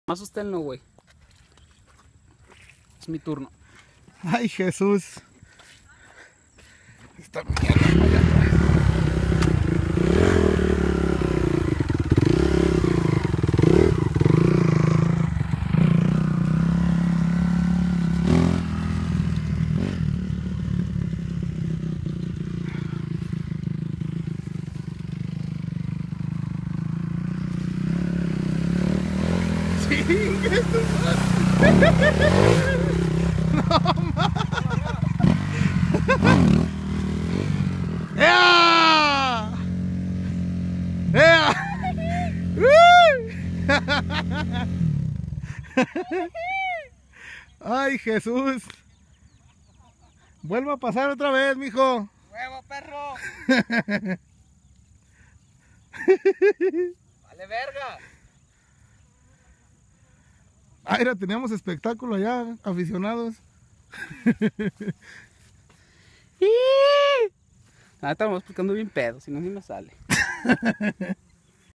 Accidente en motocross chapala
hola que tal les dejo un videito que se grabo en chapala jalisco mexico me cai por andar reparando la moto chequen esto: